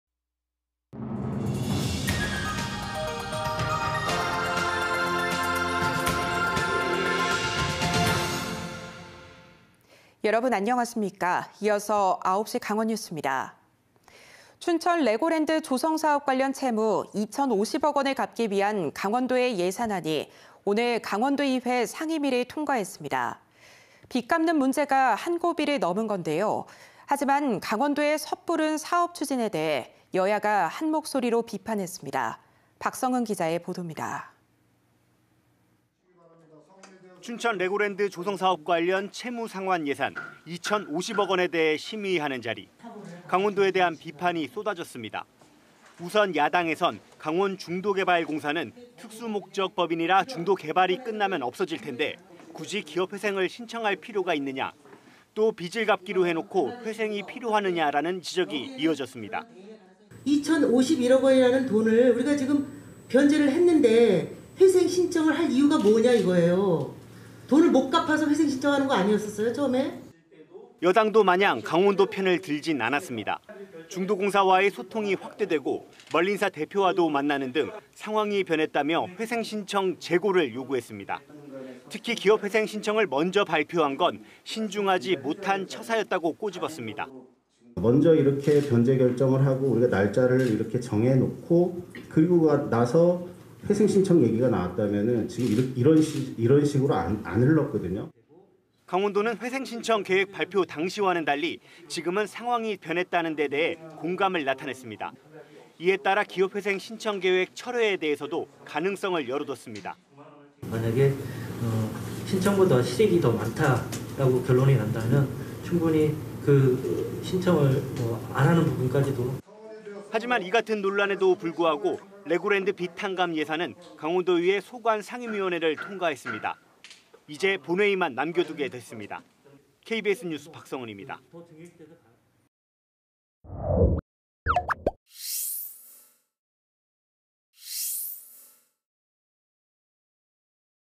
“중도공사 회생 신청 철회 요구”…강원도 “검토” / KBS 2022.11.23.
[앵커]

 춘천 레고랜드 조성 사업 관련 채무 2,050억 원을 갚기 위한 강원도의 예산안이 오늘(23일) 강원도의회 상임위를 통과했습니다.

 빚 갚는 문제가 한고비를 넘은 건데요,

 하지만 강원도의 섣부른 사업 추진에 대해 여야가 한목소리로 비판했습니다.

 박성은 기자의 보도입니다.

 [리포트]

 춘천 레고랜드 조성 사업 관련 채무 상환 예산, 2,050억 원에 대해 심의하는 자리.

 강원도에 대한 비판이 쏟아졌습니다.

 우선, 야당에서는 "강원중도개발공사는 특수목적법인이라 중도 개발이 끝나면 없어질 텐데, 굳이 기업회생을 신청할 필요가 있느냐?"

 또, "빚을 갚기로 해놓고, 회생이 필요하냐?"라는 지적이 이어졌습니다.

 [박윤미/강원도의회 의원/더불어민주당  :  "2,051억 원이라는 돈을 우리가 지금 변제를 했는데 회생 신청을 할 이유가 뭐냐 이거예요. 돈을 못 갚아서 회생 신청하는 것 아니었어요? 처음에?"]

 여당도 마냥 강원도 편을 들진 않았습니다.

 중도공사와의 소통이 확대되고, 멀린사 대표와도 만나는 등 상황이 변했다며, 회생 신청 재고를 요구했습니다.

 특히, 기업회생 신청을 먼저 발표한 건 신중하지 못한 처사였다고 꼬집었습니다.

 [김기홍/강원도의회 의원/국민의힘  :  "먼저 이렇게 변제 결정을 하고 우리가 날짜를 이렇게 정해 놓고, 그러고 나서 회생 신청 얘기가 나왔다면, 지금 이런 식으로 안 흘렀거든요."]

 강원도는 회생 신청 계획 발표 당시와는 달리, 지금은 상황이 변했다는 데 대해 공감을 나타냈습니다.

 이에 따라 기업회생 신청 계획 철회에 대해서도 가능성을 열어뒀습니다.

 [윤인재/강원도 산업국장  :  "만약에 (기업회생) 신청보다 실익이 더 많다라고 결론이 난다면, 충분히 그 신청을 안 하는 부분까지도…."]

 하지만 이 같은 논란에도 불구하고 레고랜드 빚 탕감 예산은 강원도의회 소관 상임위원회를 통과했습니다.

 이제 본회의만 남겨두게 됐습니다.

 KBS 뉴스 박성은입니다.

 촬영기자:이장주